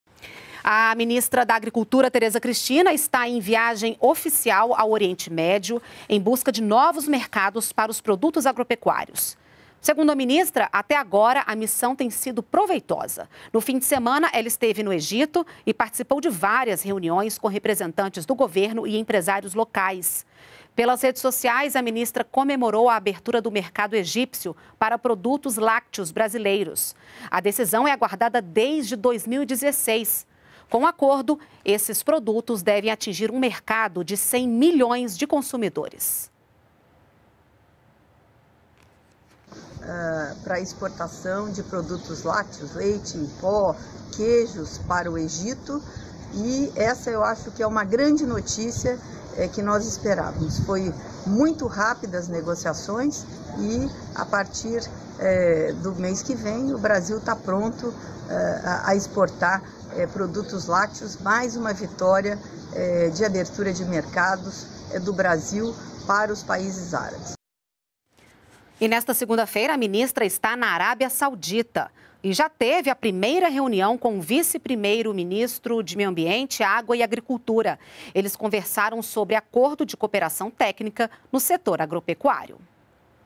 Ministra Tereza Cristina apresenta agronegócio brasileiro ao Oriente Médio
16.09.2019 - A ministra da Agricultura está em viagem oficial ao Oriente Médio, em busca de novos mercados para os produtos agropecuários.